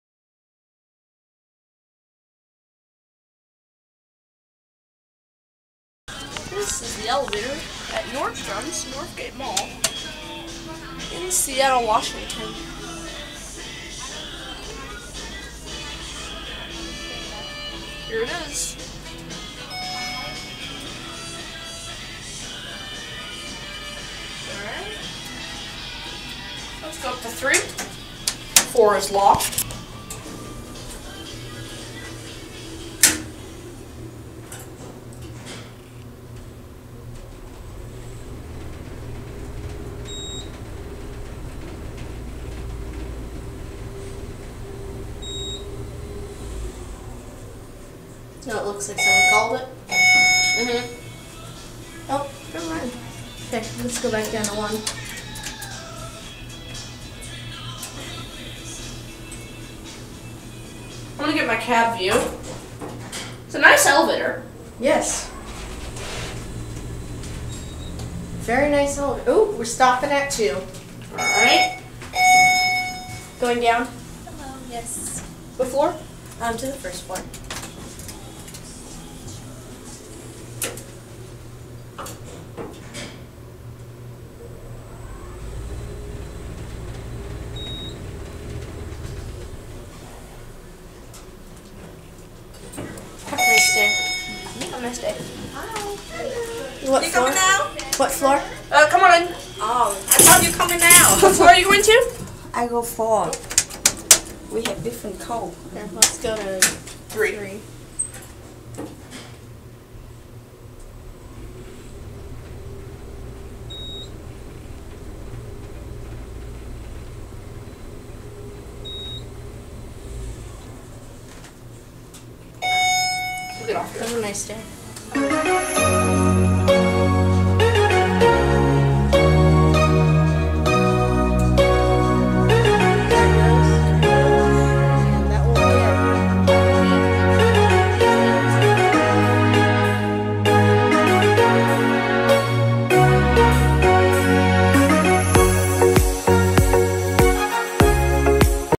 CLOSING TODAY! FINAL RIDE! Montgomery Kone Elevator At Nordstrom Northagte mall Seattle WA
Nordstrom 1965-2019
I will all ways remember this store i hope they do not Gut this elevator notice that you can hear the motor!
I HAVE HEARD THIS STORE HAS BEEN HAVING BLACKOUTS LATELY SO PEOPLE HAVE BEN GETTING STUCK.

Specs
Installed by- Montgomery
Drive system- Traction
Type- Overhead
Fixtures- Innovation Brewser
Door equipment- Mac

feedback about my videos and recommendations.
Filmed on iPhone Crap camera
Location filmed- Nordstrom Northgate mall seattle wa
Date filmed- May 2019